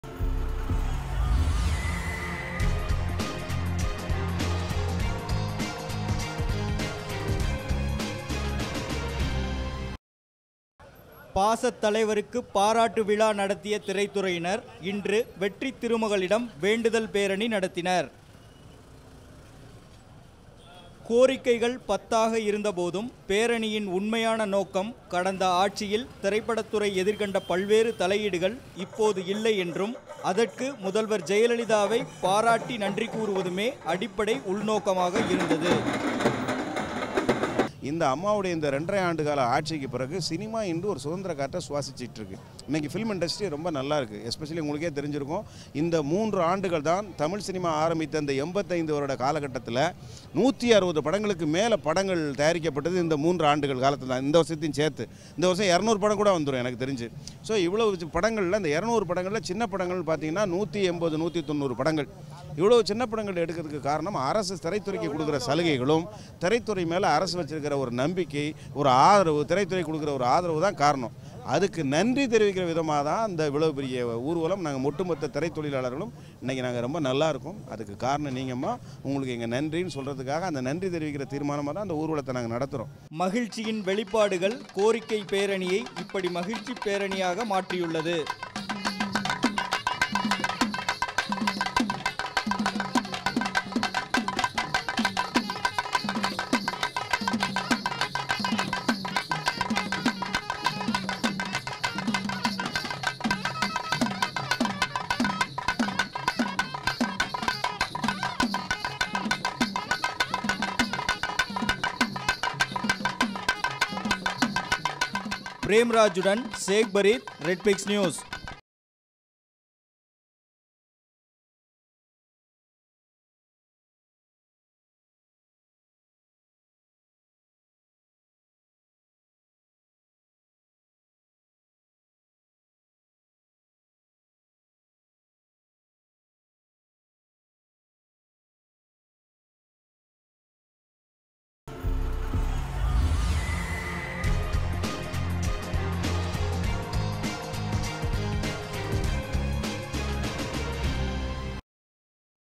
The Real truth behind South India Film Federation's Mega Rally -- Red Pix
Film Employees' Federation of South India (Fefsi), a collective of 23 independent film technicians' unions, is asking the Tamil Nadu government to revive Film City in Taramani or elsewhere in order to facilitate Tamil films to be canned at locations within the State.

On Monday, Fefsi president and popular director, Ameer, unveiled the Federation's new flag and listed the demands they would be placing with the Chief Minister's office on Tuesday at the end of a proposed rally.

Apart from the demand to revive Film City, Ameer said the Federation would also request the Chief Minister to provide for tax concessions for film shootings in the State.

"This would prompt producers to shoot large portions of their films in the State, and this would help local technicians," Ameer said. "Currently most films are shot in neighbouring States, where there are strict rules on compulsorily engaging local technicians," he said.

The Film City project in Taramani was originally launched by the Tamil Nadu government in the 1990s and thrived as a shooting spot for several films.

However, in recent years, it has fallen to disuse and more recently, large portions of its land have been handed over to Tamil Nadu Industrial Development Corporation to be converted into a software park.

The Federation will also request the CM to provide tax concessions for filming in the State